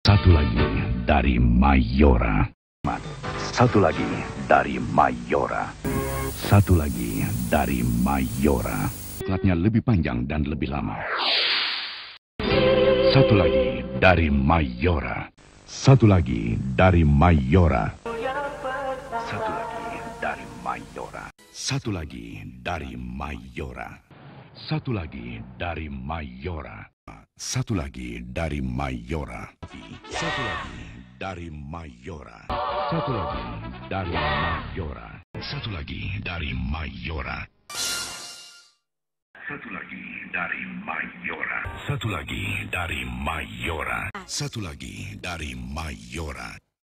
Mayora Logo Sound History New Updated
This logo sound history for development food product, I could be proud back in indonesia comes away for the new logo compilation is coming! That's it, it's a indonesian food product was right?

Inspired by Kurniady Saputra
Recorded by Bandicam